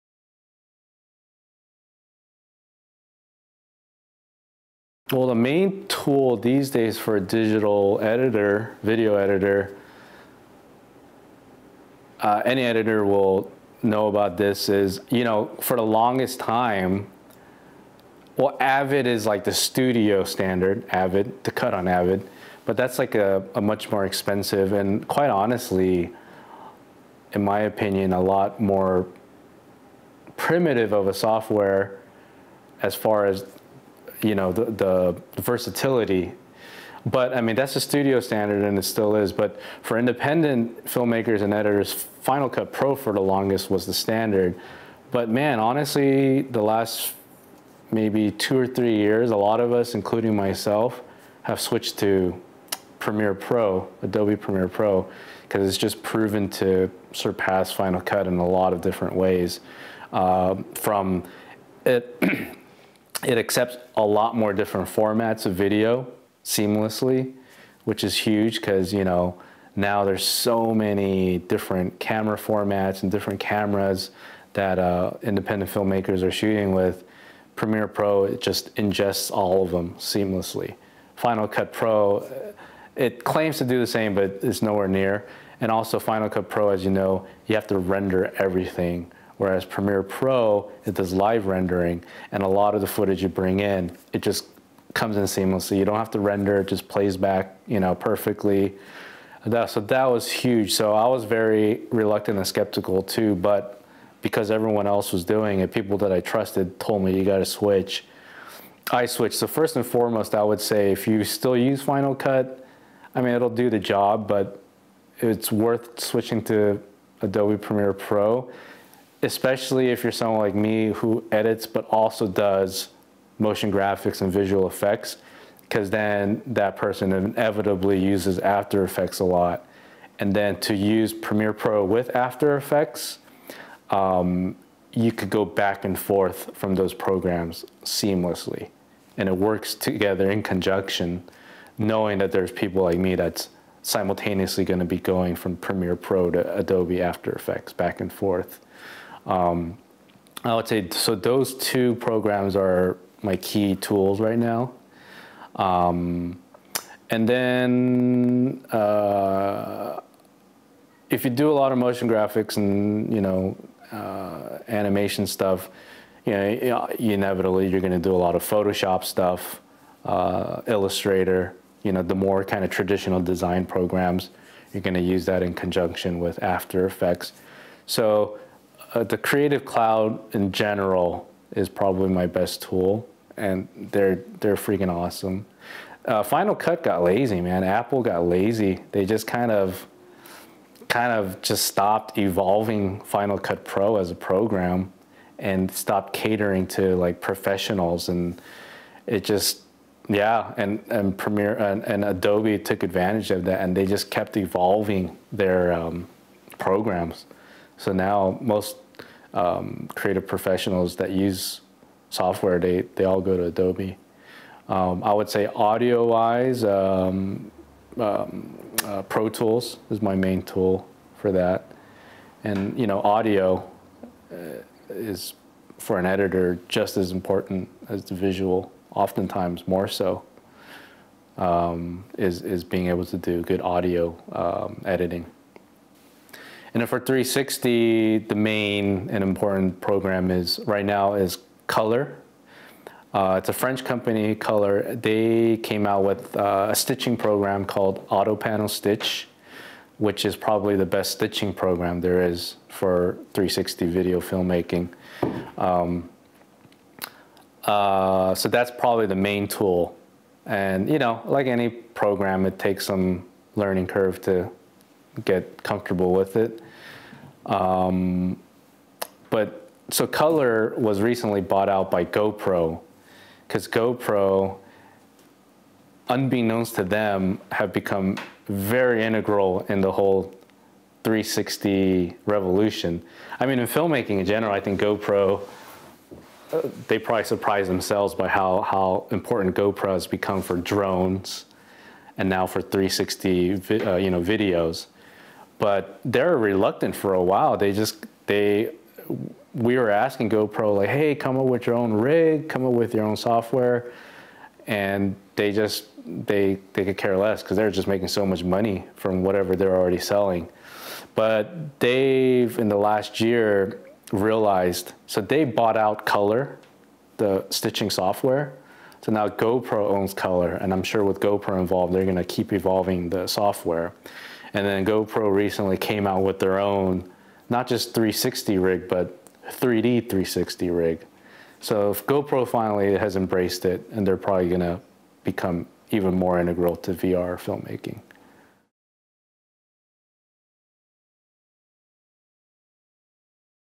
What Software Do You Use For Post-Production?
Jimmie Rhee, video editor and visual effects specialist, discusses the software tools that he uses for editing film, visual effects, and 360° video.